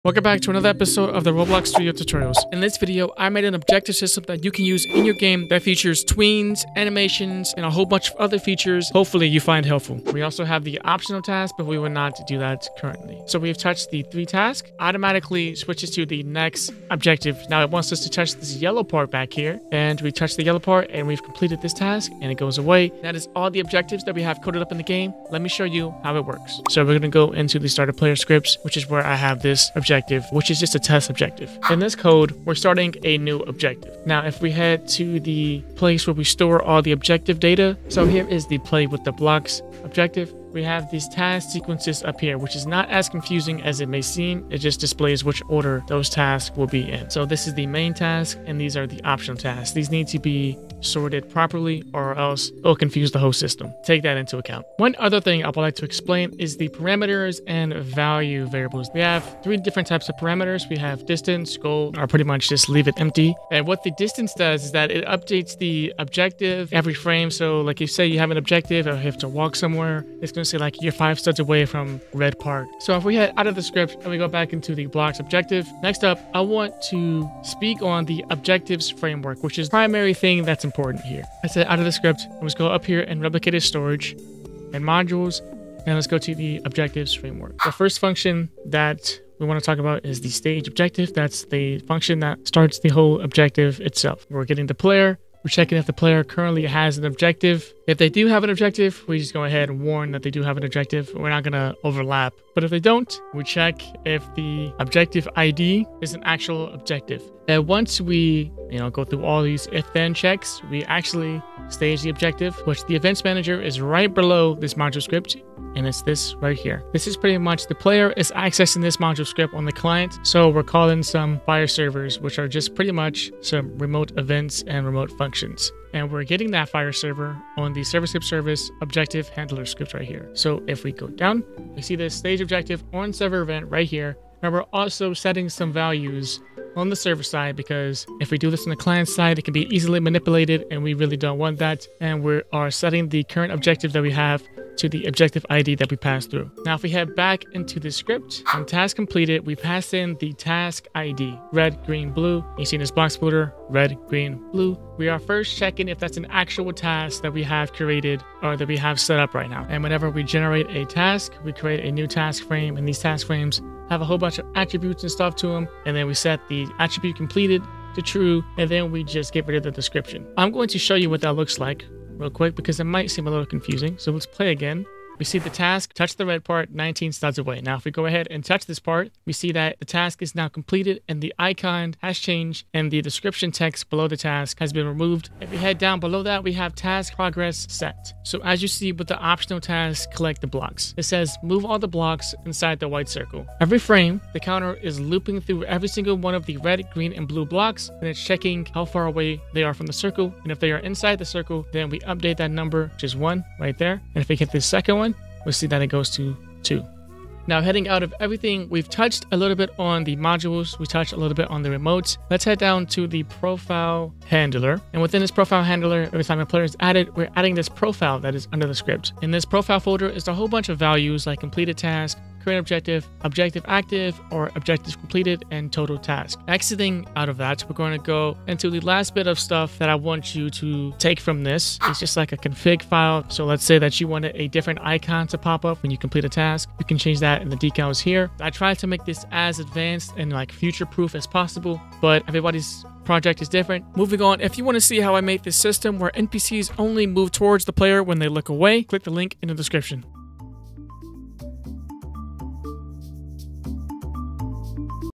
Advanced Objective System (Roblox Studio Tutorial)
Learn how to build a simple objective system in Roblox Studio in this video!